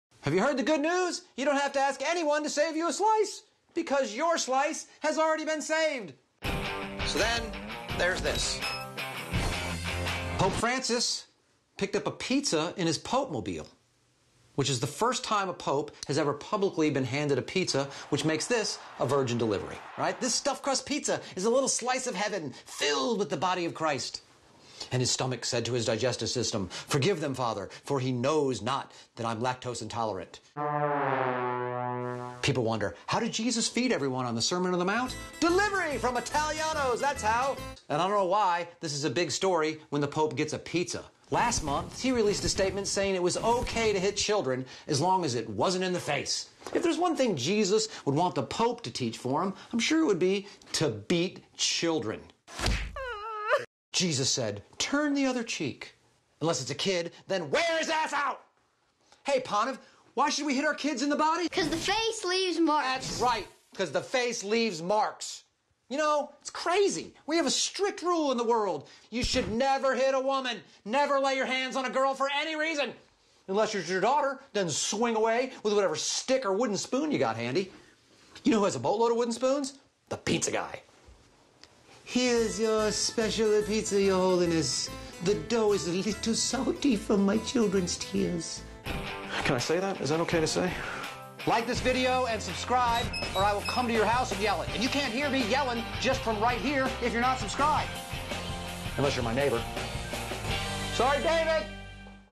The Pope Getting Pizza Isn't What We Should Talk About
According to the Pope, what do pizza dough and kids have in common? They both could use a beating. Got any thoughts on the topic? Let us know using THISwithTom.